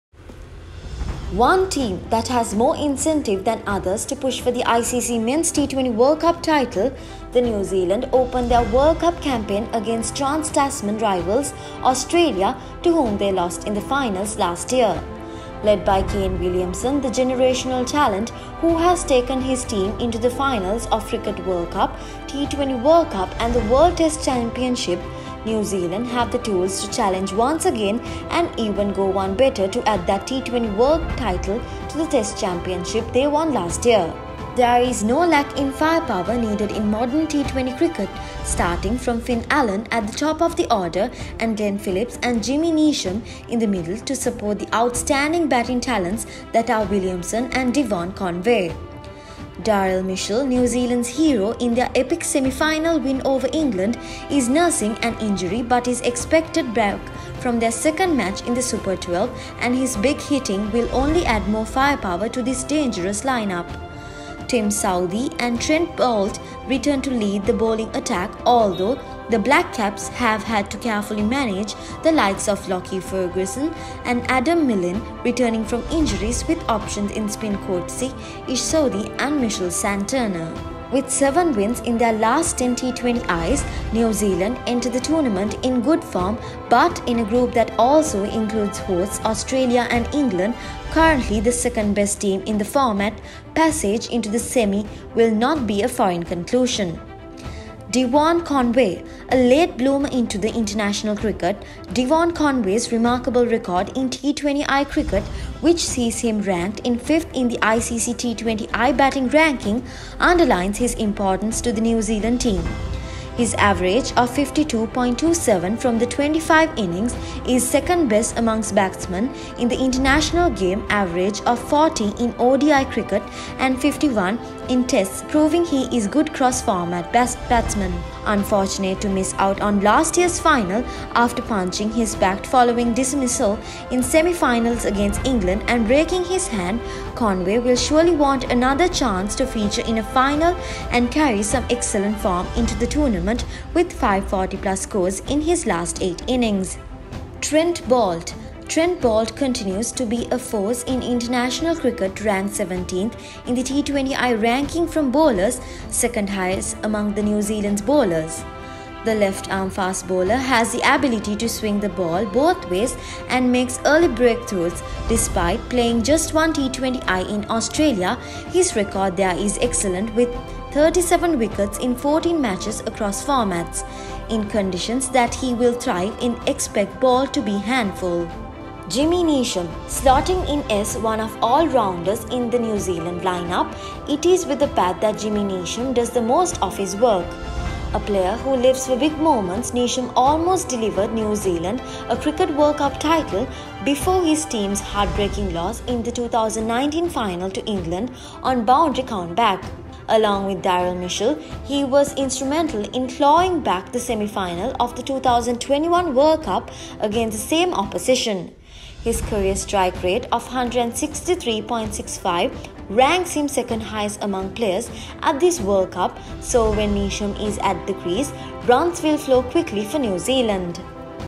ICC Men's T20 World Cup 2022 - Team Preview - New Zealand
Last year's beaten finalists New Zealand will hope to go one better, with new additions to the squad that almost went the distance in 2021.

Powered by: HNB